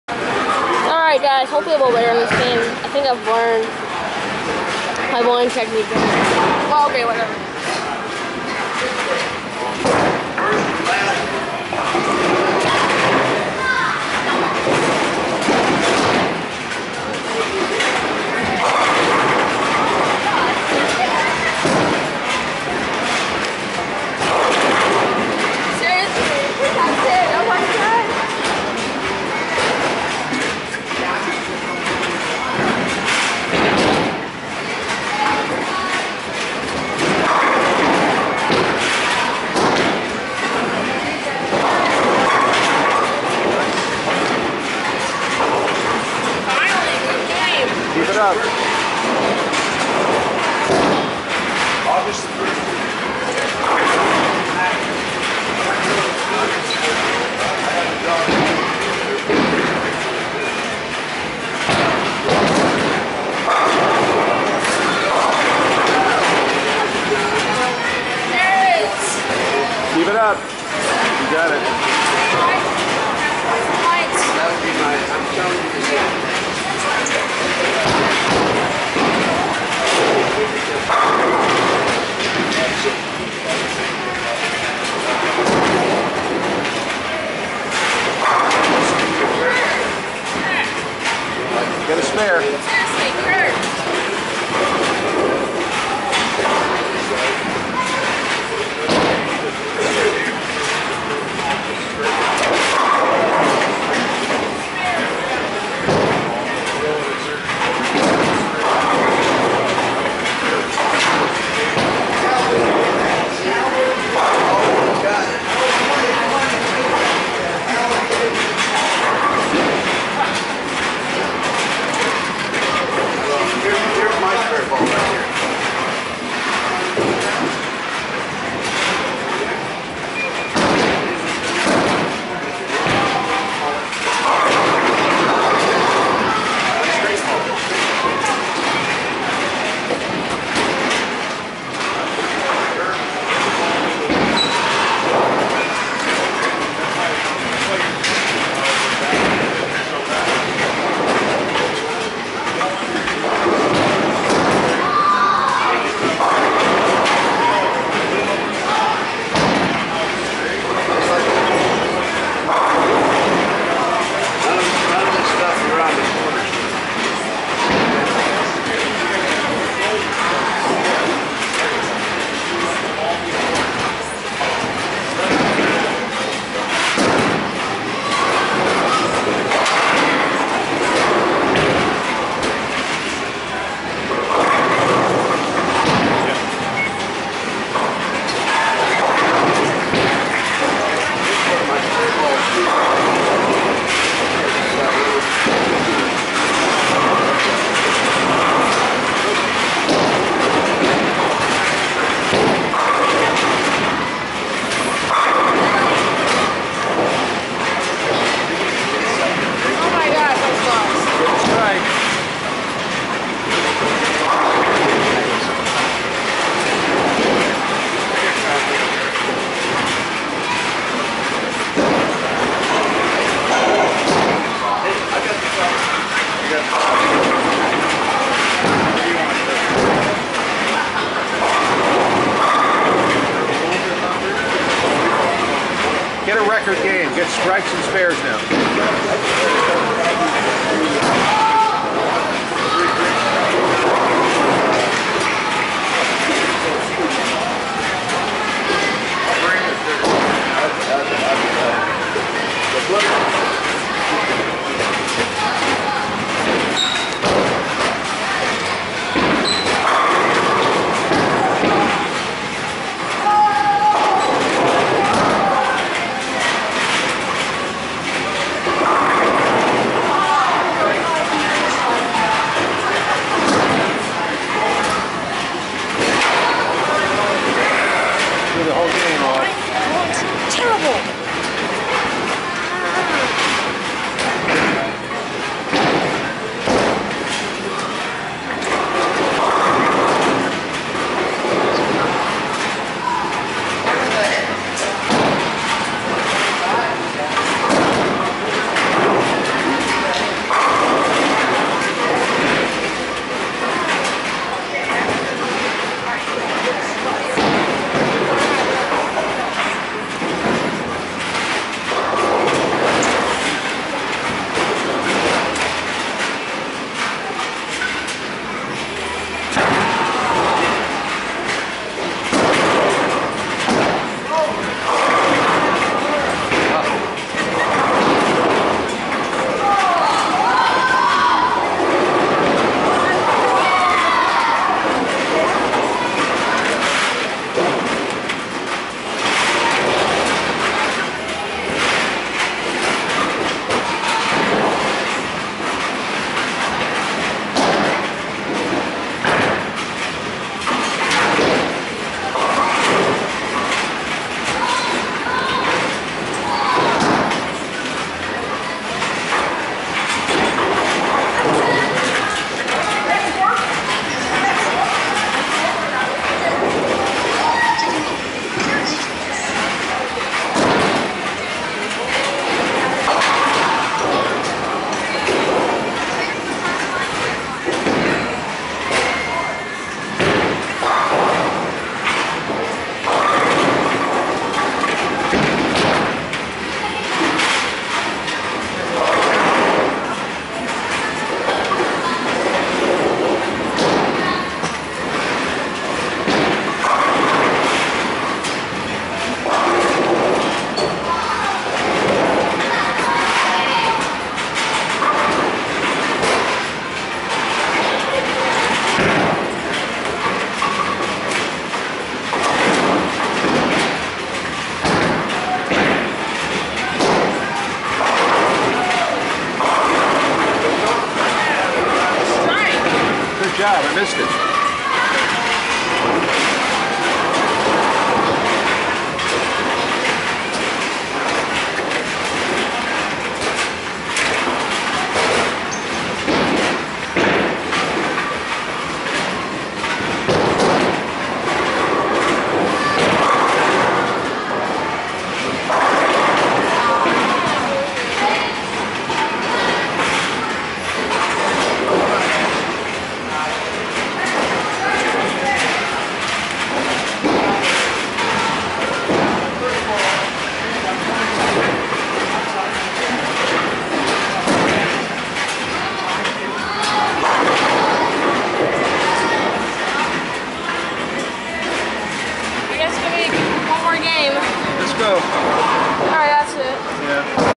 Bowling at Sunset Station Part 3/4: New Camera Angle!
Well you can clearly see my footwork and my throw with this camera angle.